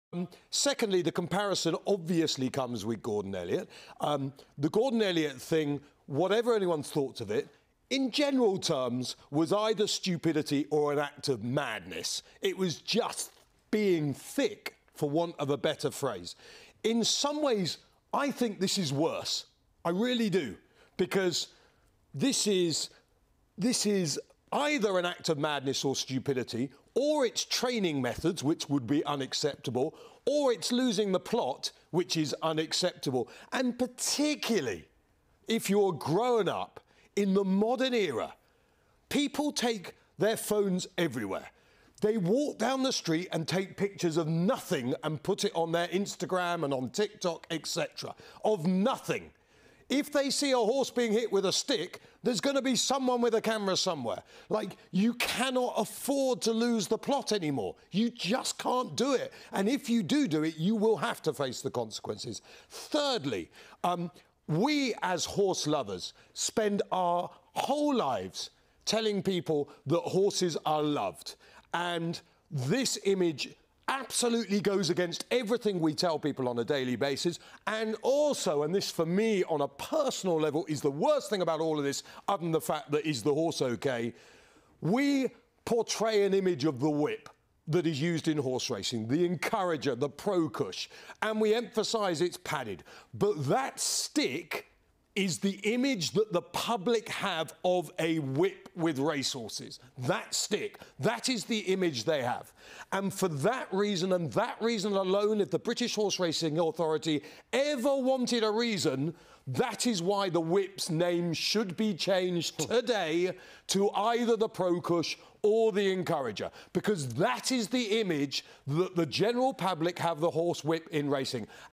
"It's worse than the Gordon Elliott photo" - Matt Chapman on the Sir Mark Todd video
Matt Chapman weighed in on the story surrounding Sir Mark Todd and a video which has emerged of the trainer and equestrian legend whipping a horse with a branch.